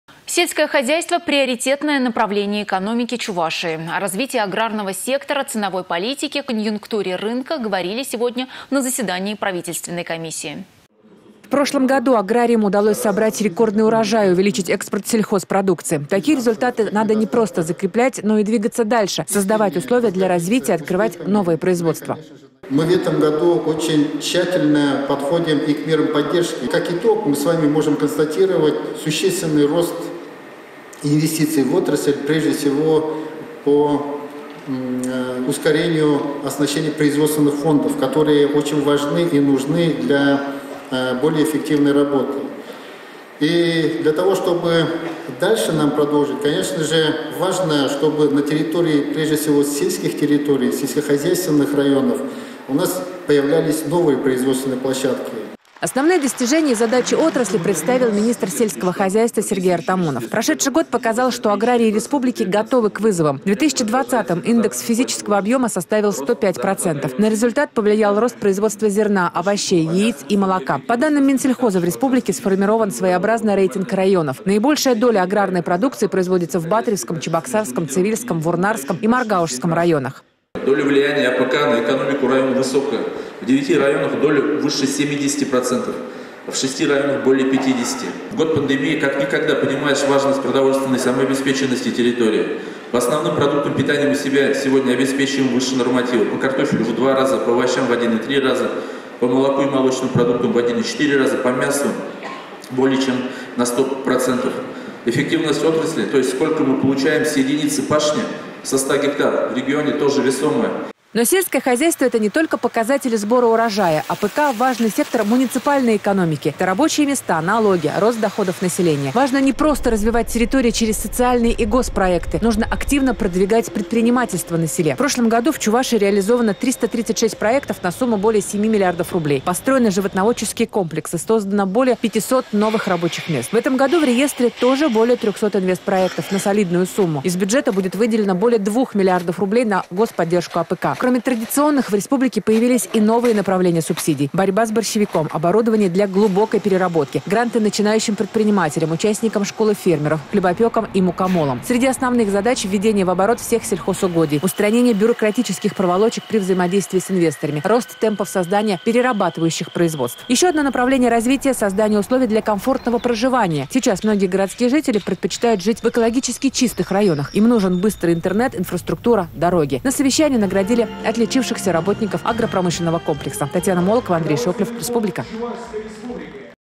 Сельское хозяйство — приоритетное направление экономики Чувашии
О развитии аграрного сектора, ценовой политике, конъюнктуре рынка  говорили сегодня на заседании правительственной комиссии.